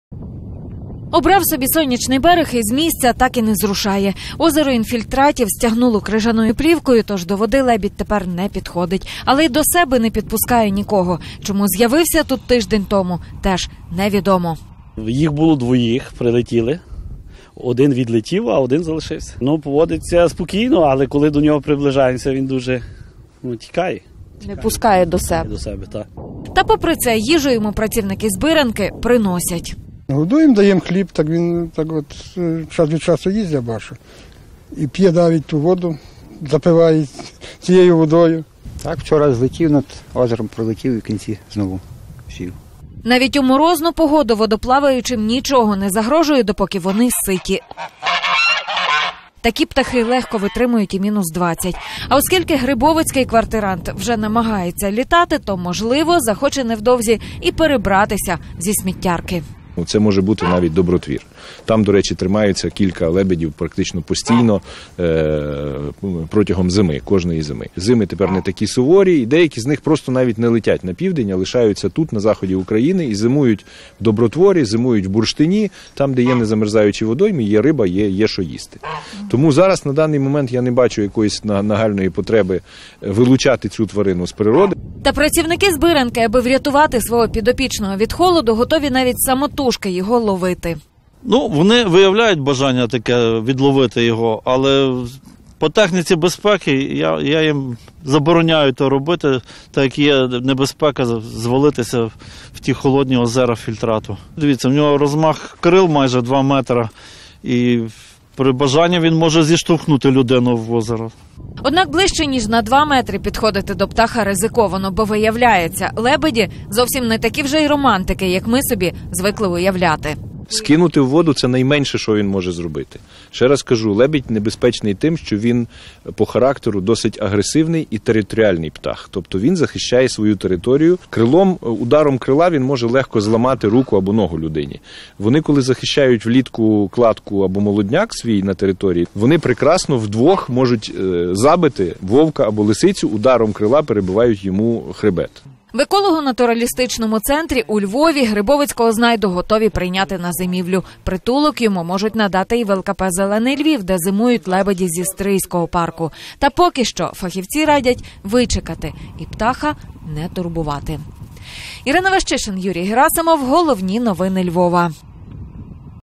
На Грибовицькому сміттєзвалищі у Львові поселився лебідь
Уже тиждень лебідь-шипун мешкає біля озера інфільтратів, яке утворилось поміж горами сміття на Грибовицькому полігоні, що біля Львова. Дотепер він навіть плавав у воді, яку вважають токсичною. Але зараз, коли на поверхні з’явився лід, його перебування там багатьом видається небезпечним.

До себе птах не підпускає нікого. Чому з’явився на полігоні – теж невідомо. «Їх прилетіло двоє. Один відлетів, а один залишився. Поводиться спокійно, але коли ми наближаємося, тікає. Не підпускає до себе», – розповів працівник ЛКП «Збиранка» Віталій Куприч.

Та попри це їжу йому люди приносять. «Годуємо, даємо хліб, так він час від часу їсть, я бачу. І п’є навіть цю воду з озера, запиває. Вчора злетів, над озером пролетів і потім знову сів», – спостерігали за птахом працівники ЛКП «Збиранка» Микола Мороз і Степан Мигул. Навіть у морозну погоду диким водоплаваючим нічого не загрожує, допоки вони ситі, запевняють екологи. Такі птахи легко витримують і -20 градусів. А оскільки грибовицький квартирант вже намагається літати, то, можливо, захоче невдовзі й перебратися зі сміттярки, – наголосив еколог та керівник дитячого еколого-натуралістичного Центру Ігор Антахович. «Це може бути навіть Добротвір. Там, до речі, декілька лебедів є постійно, кожної зими. Зими тепер не такі суворі, і деякі з птахів просто не летять на південь, а лишаються на заході України і зимують в Добротворі, у Бурштині, де є незамерзаючі водойми, де є риба. Тому зараз я не бачу якоїсь нагальної потреби вилучати цього лебедя з природи», – додав Ігор Антахович. Та працівники Збиранки, аби врятувати свого підопічного від холоду, готові навіть самотужки його зловити. «Вони виявляють таке бажання – відловити. Але за технікою безпеки я їм це забороняю, оскільки є небезпека звалитися в ті холодні озера інфільтрату. В нього ж розмах крил майже два метри і при бажанні він може зіштовхнути людину в озеро», – застеріг директор ЛКП «Збиранка» Володимир Занік.

Однак ближче ніж на два метри підходити до птаха ризиковано, бо, виявляється, лебеді – зовсім не такі романтики, як ми собі звикли уявляти. «Скинути у воду – це найменше, що він може зробити. Лебідь за характером досить агресивний і територіальний птах. Тобто він захищає свою територію і в нього є межа, за яку він просто не підпускає до себе. Його основна зброя – це крила. Ударом крила він може легко зламати руку або ногу людині. Вони, коли захищають влітку кладку яєць або свій молодняк, удвох можуть забити вовка або лисицю. Тобто ударом крила вони перебивають хребет», – розповів Ігор Антахович. В еколого-натуралістичному центрі у Львові грибовицького знайду готові прийняти на зимівлю. Притулок йому можуть надати і в ЛКП «Зелений Львів», де щороку зимують лебеді зі Стрийського парку. Та поки що фахівці радять вичекати і птаха на сміттєзвалищі не турбувати.